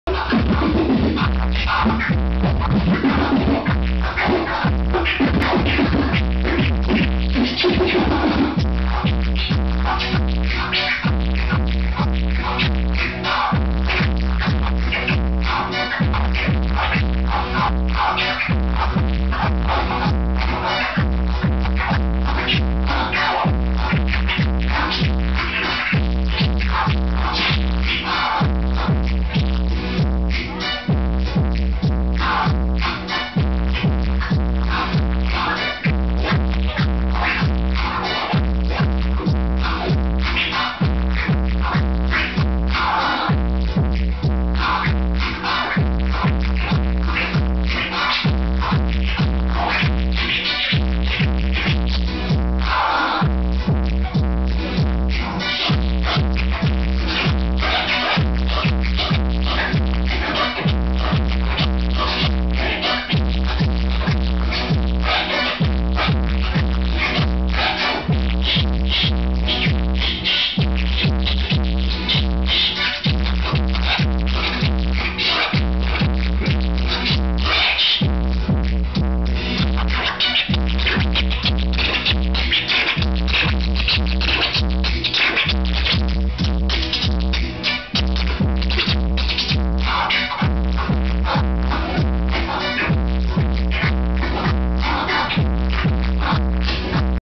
DJs Speed, Non-stop, Flipside... scratch @ B96 Summer Bash
DJ Nonstop (DMX's DJ), DJ Speed, DJ Flipside & DJ Mark E... scratch on the turntables, in consecutive order. This took place after the concert started, so, several people missed out on the scratch session. Lots of bass messes up the audio, but, still cool to see.